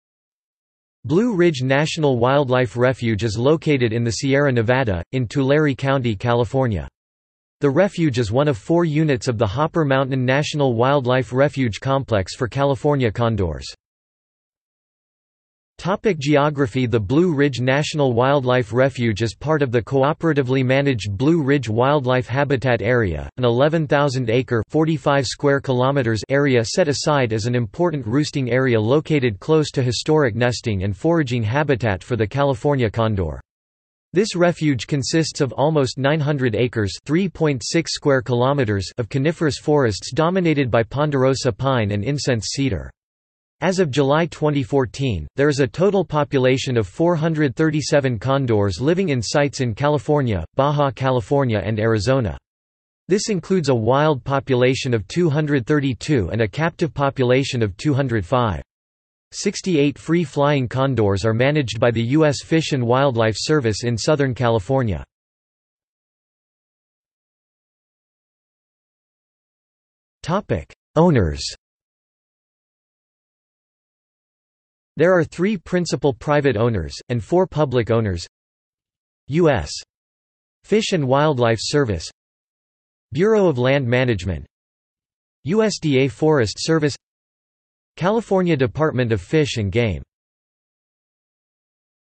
Blue Ridge National Wildlife Refuge | Wikipedia audio article
This is an audio version of the Wikipedia Article:
Blue Ridge National Wildlife Refuge

00:00:15 1 Geography
00:01:06 1.1 Owners

- Socrates

SUMMARY
Blue Ridge National Wildlife Refuge is located in the Sierra Nevada, in Tulare County, California. The refuge is one of four units of the Hopper Mountain National Wildlife Refuge Complex for California condors.